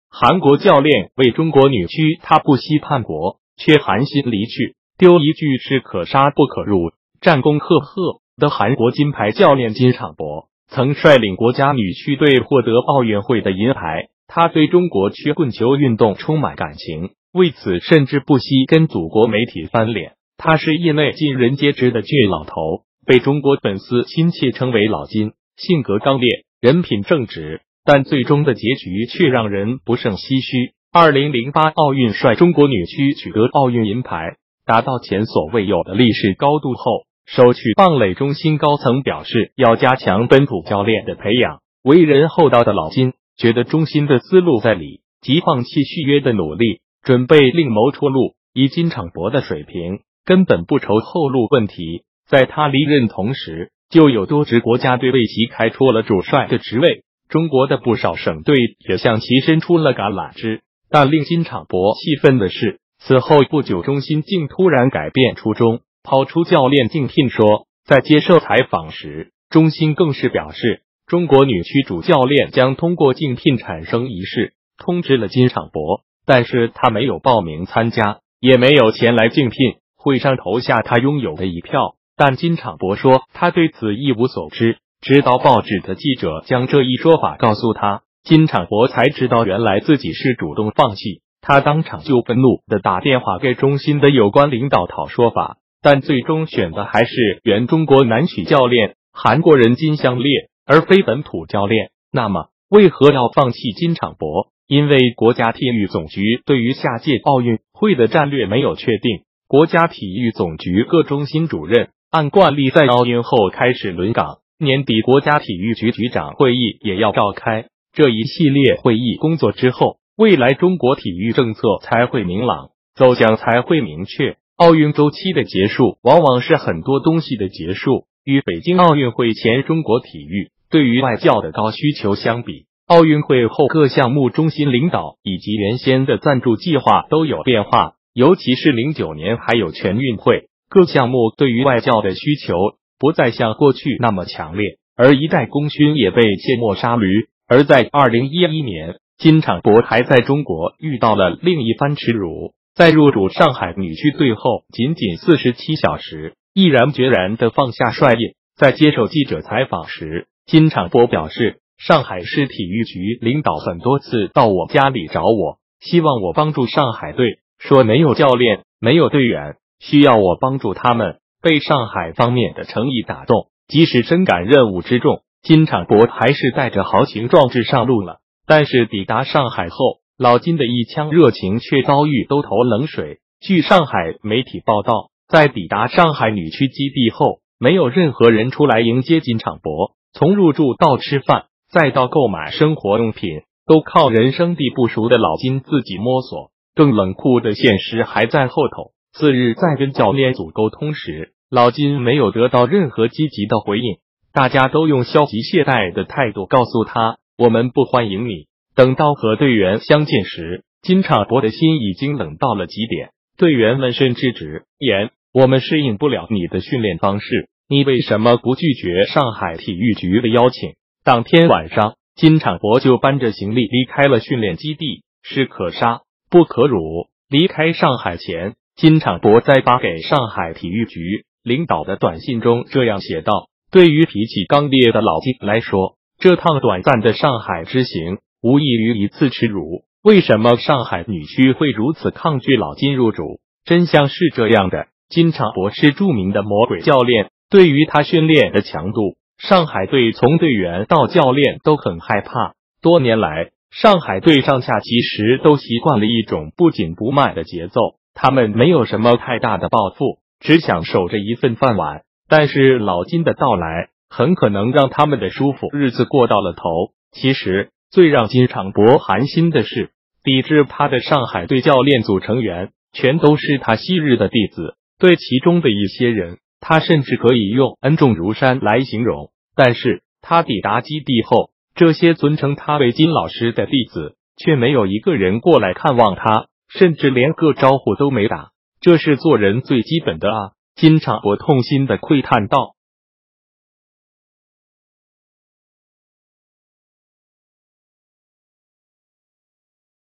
韓國教練為中國女曲他不惜叛國，卻寒心離去，丟一句“士可殺不可辱”
他對中國曲棍球運動充滿感情，為此甚至不惜跟祖國媒體翻臉；他是業內盡人皆知的“倔老頭”，被中國粉絲親切稱為“老金”，性格剛烈，人品正直，但最終的結局卻讓人不勝唏噓。